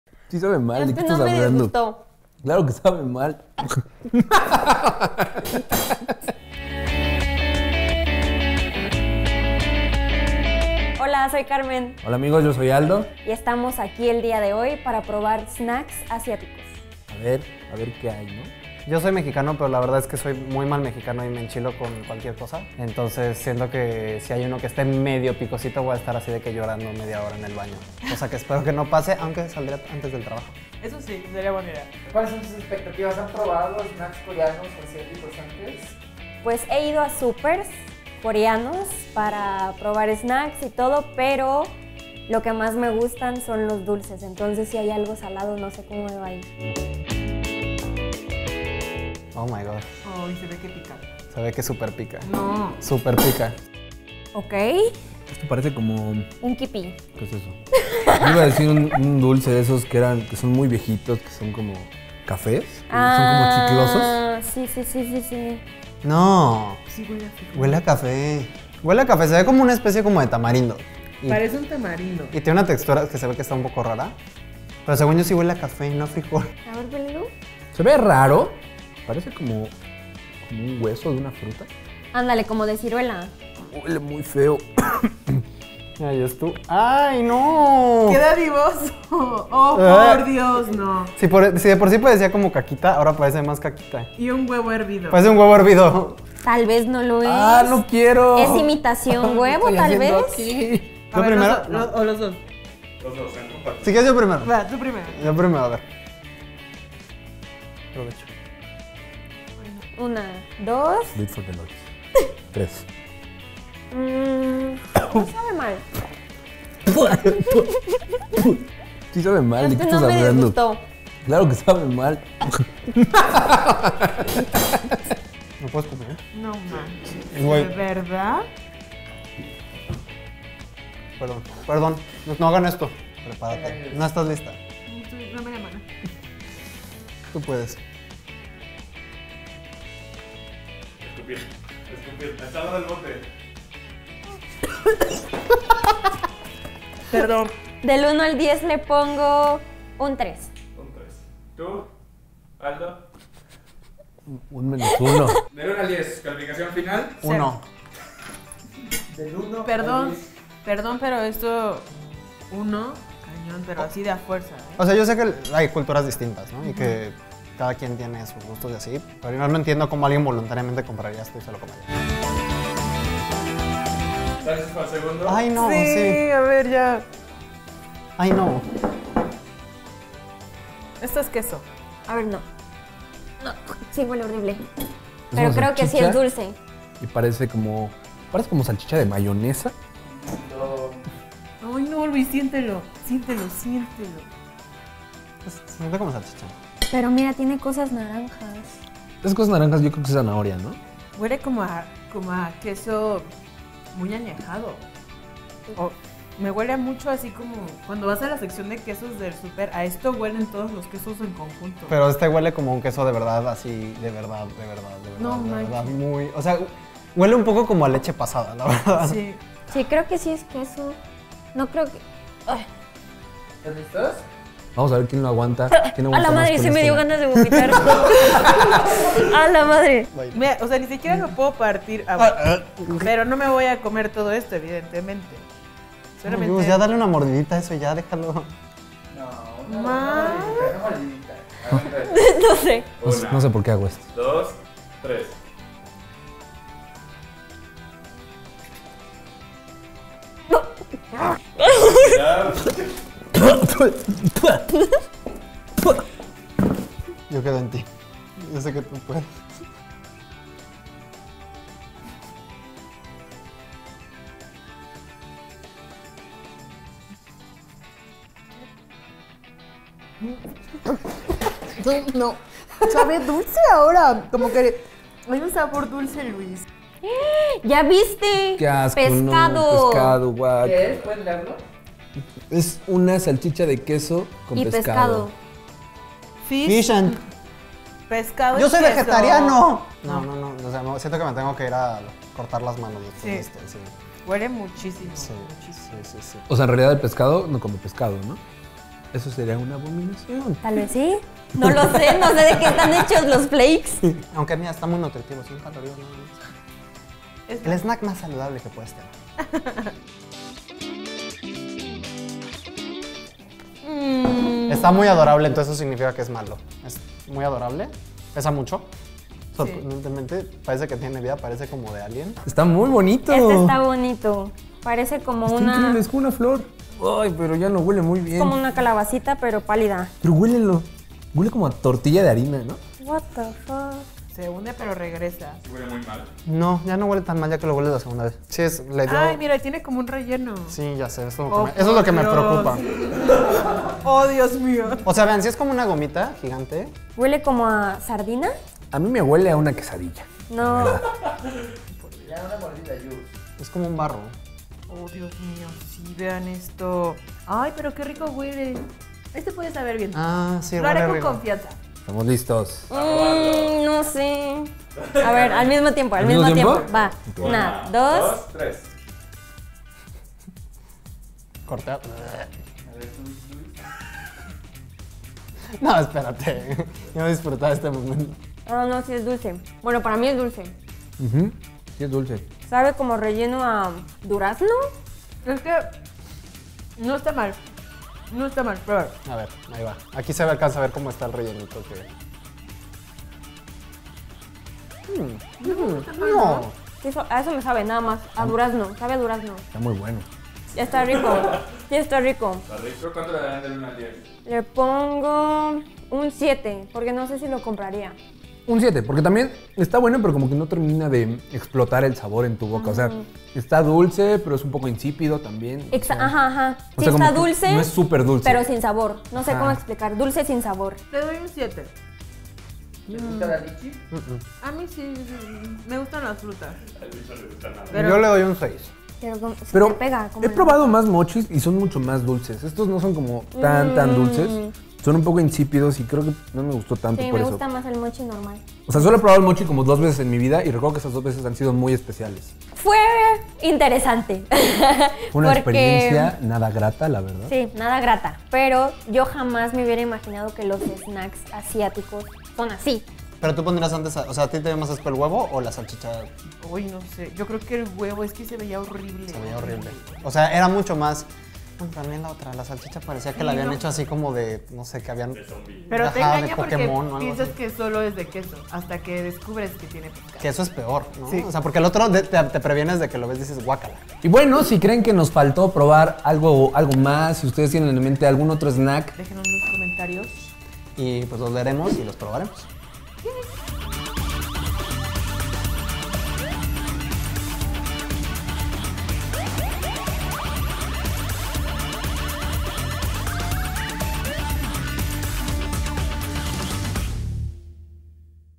Mexicanos prueban botanas asiáticas
¿Alguna vez has tenido ganas de probar muchos dulces asiáticos pero no te atreves? ¡Nosotros nos arriesgamos por ti! En el menú del día de hoy contamos con botanas que incluyen pescado, queso, chile y... ¿lichi?